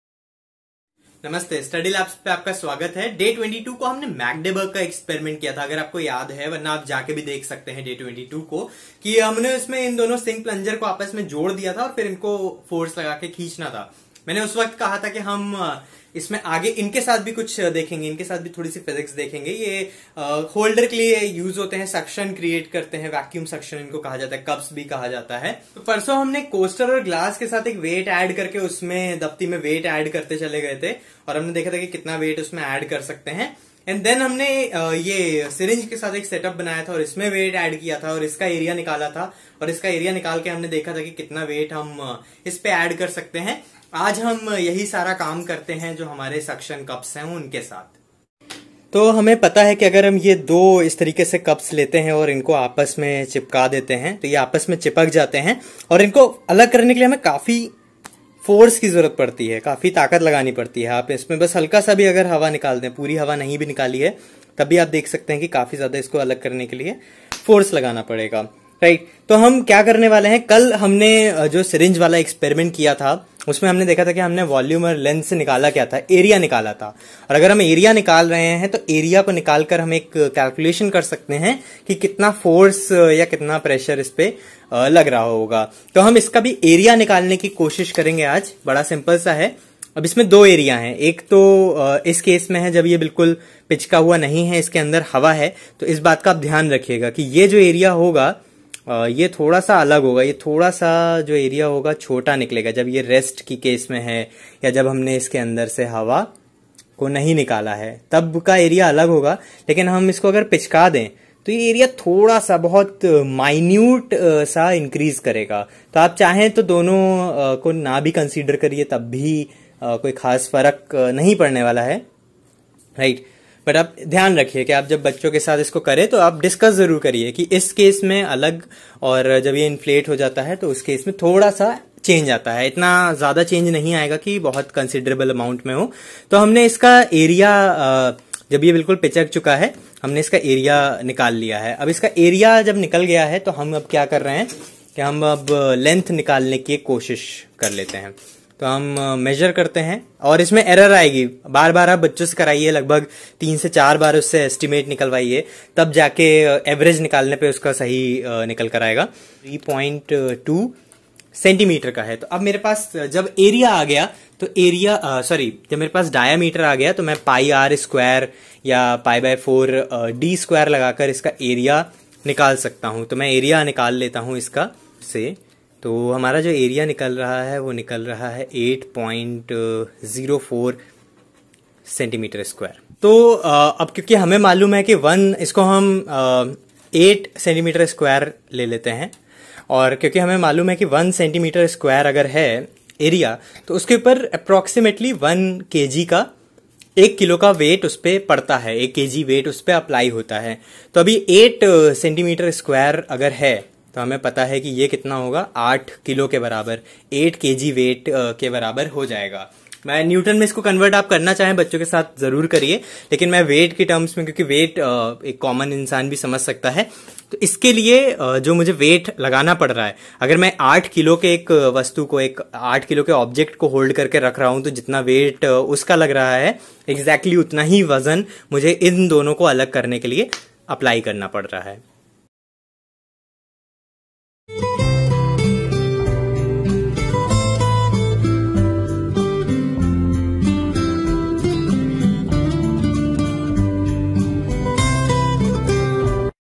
#52 How much force to separate two vacuum cups?(वैक्यूम कप पर कितना बल लगता हैं)-Pressure Experiment
A journey of 1000 Experiments